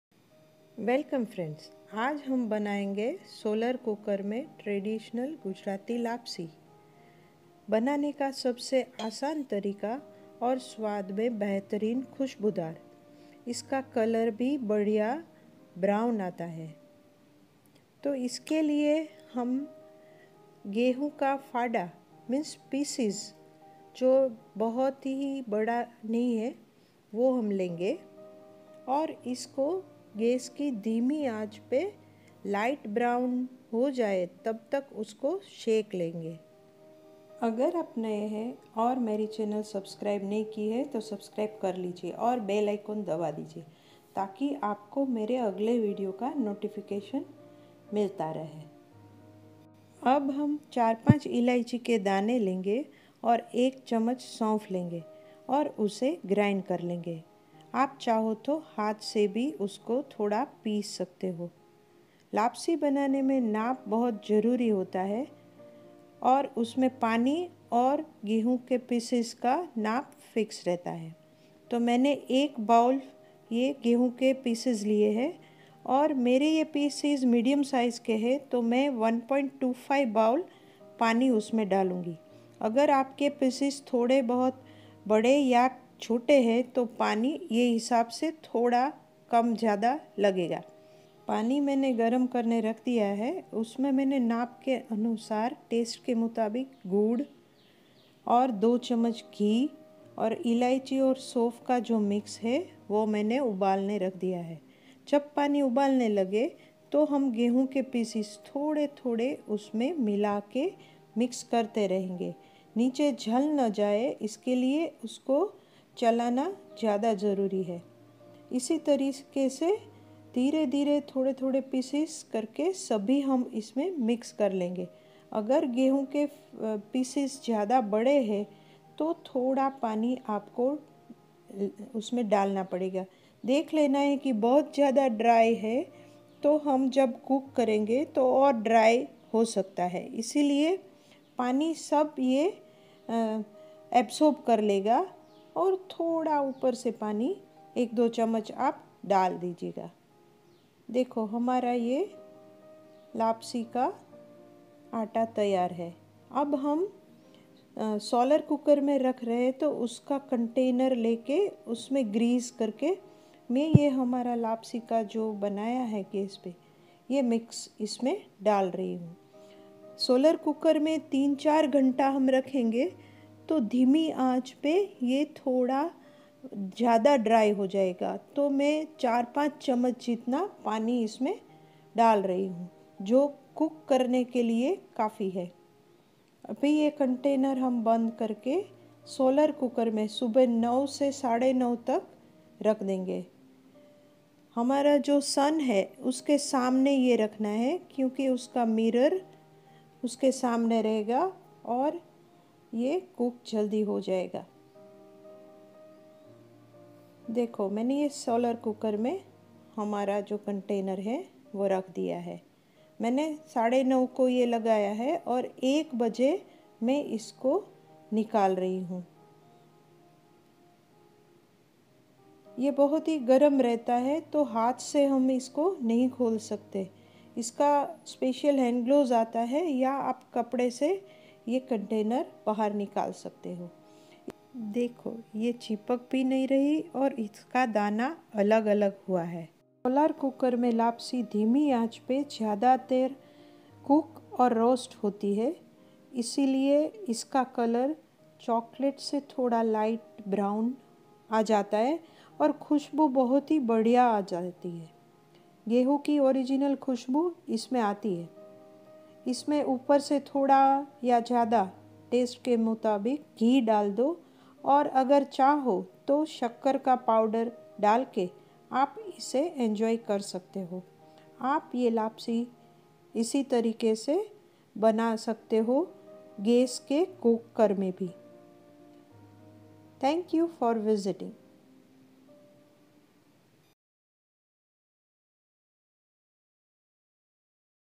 Gujarati Lapsi recipe in Hindi | Broken wheat | ગુજરાતી છુટ્ટી ફાડા લાપસી | Solar cooker recipes
Make this easy Gujarati lapsi in solar cooker. Perfect for any festive occasion or as a dessert in any meal.

Ingredients:
Broken Wheat pieces/ fada / dalia : 1 bowl
Water : 1.25 bowl
(Always take 1:1.25 ratio of dalia:water)
cardamoms : 4 to 5
sauf/aniseeds - 1.5 to 2 tbsp
Jaggery - as per taste
Pure Ghee - 2 tbsp
Cooking time: 4 to 5 hours in the sun (Keep it in the solar cooker at around 9:30 am)

I hope after seeing this Gujarati Broken wheat Lapsi recipe in Hindi, you make it yourself. ગુજરાતી છુટ્ટી ફાડા લાપસી આ નવી પદ્ધતિથી ઘરે બનાવો. This is one of the many Easy Solar cooker recipes that you MUST try at home especially during festivals.